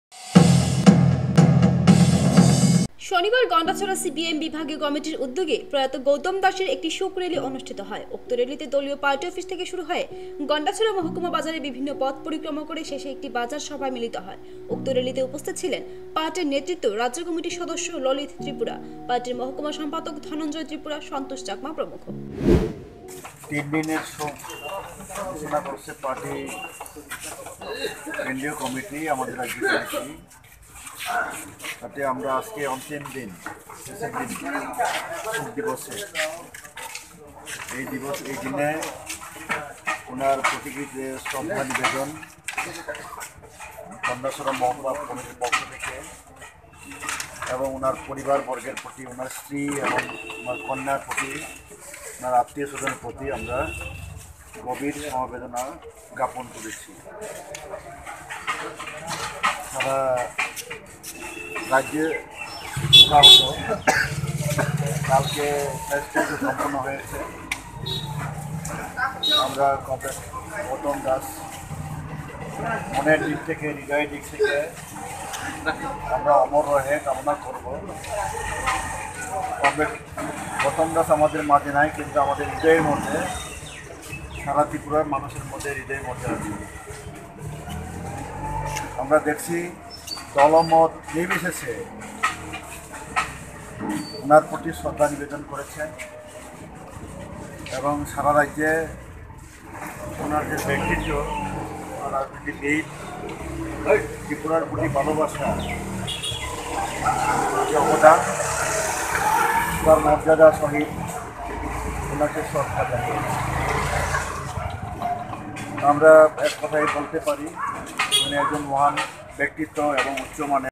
প্রয়াত গৌতম দাস এর স্মৃতির উদ্যেশে শোক রেলি গন্ডাছড়ায় || VOICE 18
VOICE 18
Name of Production House: FLAXEN MEDIA WORKS

শনিবার  গন্ডাছড়া সিপিআইএম বিভাগীয় কমিটির উদ্যোগে প্রয়াত গৌতম দাস এর একটি শোক রেলী অনুষ্ঠিত হয় উক্ত রেলীতি দলীয়  পার্টি অফিস থেকে বের হয়ে গন্ডাছড়া মহাকুমা বাজারের বিভিন্ন পথ পরিক্রমা করে শেষে একটি বাজার সভায় মিলিত হয় উক্ত রেলিতে উপস্থিত ছিলেন  পাটি নেতৃত্ব রাজ্যা কমিটি সদস্যI ললিত এিপুরা,পাটি মহকুমা সম্পাদক ধন্নজয় এিপুরা,সস্তোষ চাকমা প্রমুখ।